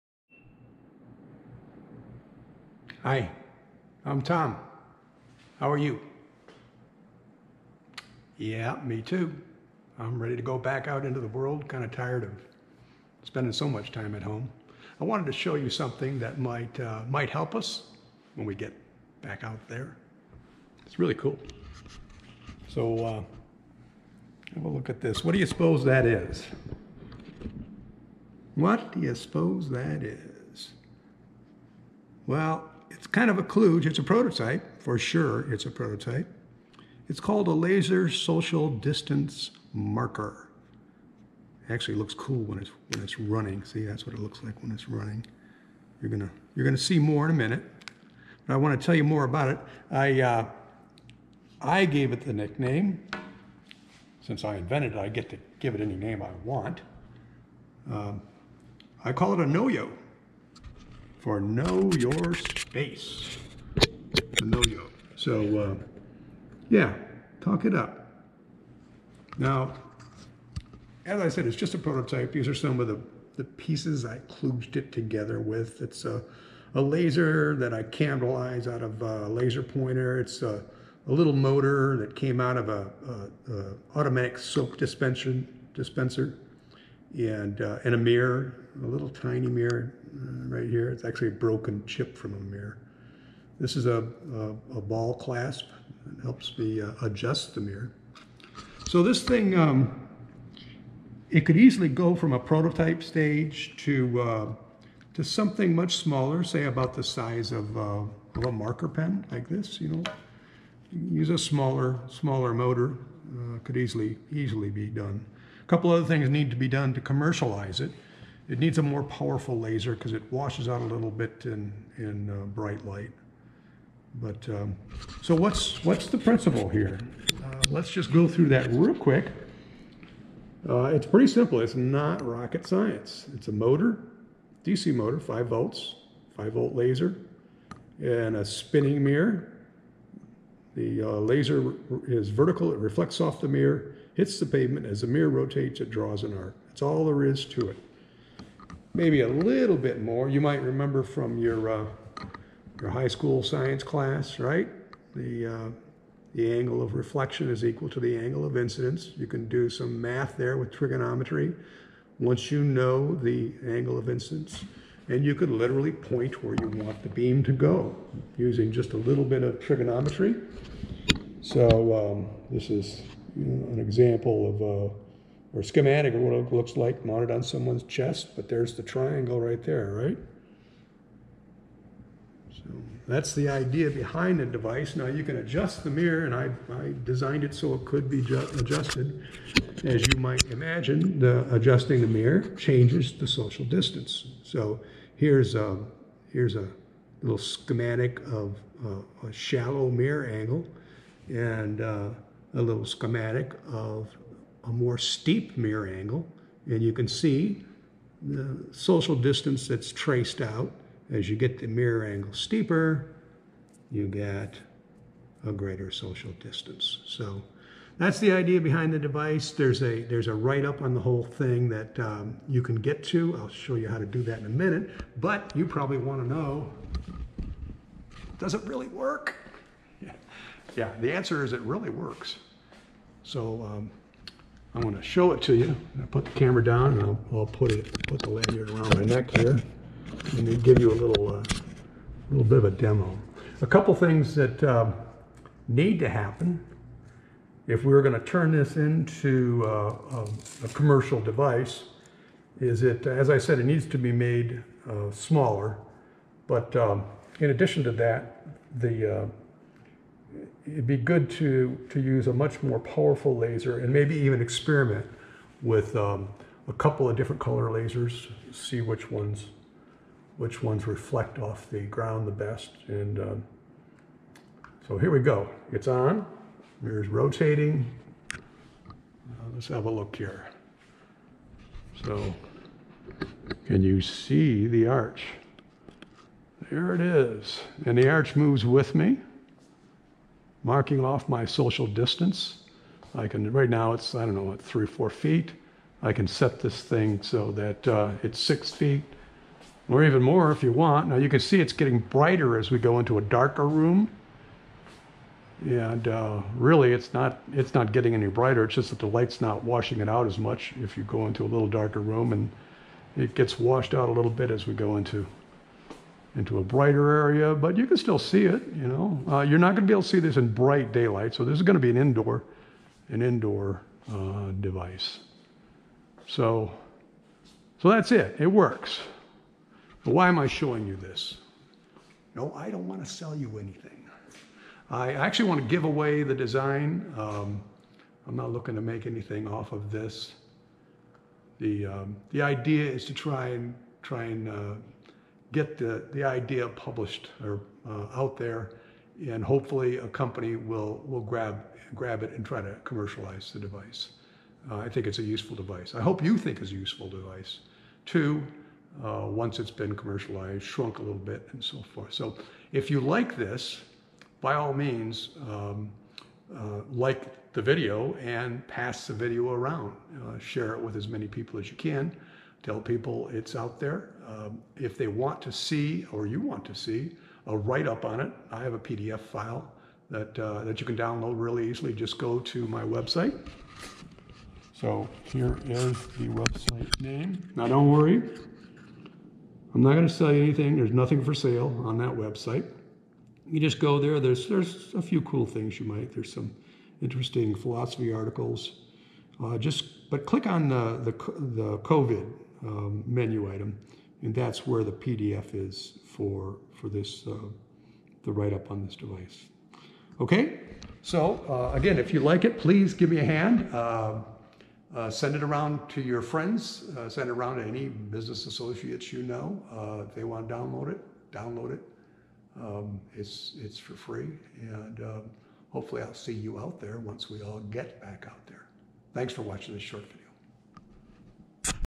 Tom's No! Yo! anti COVID
This video demonstrates the "No! Yo!" prototype – a Laser Social Distance Marker (LSDM). The designer wants to donate the intellectual property to combat COVID-19.  So, share this with as many people as you can. Let's convince one or more companies to commercialize it!